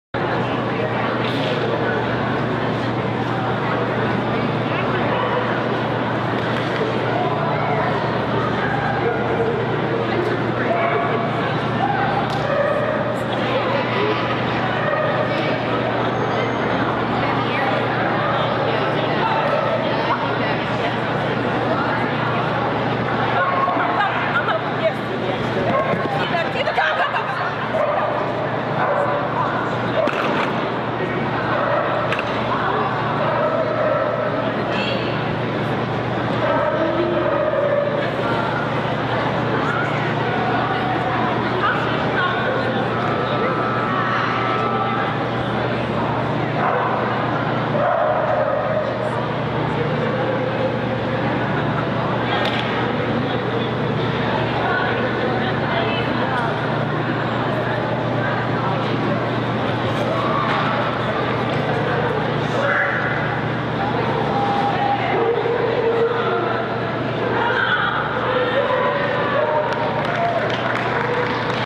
AKC Agility Alisa ridgeback Q run STD EXC B
AKC Agility Oriole Dog Training club , MD Alisa ridgeback STD EXC B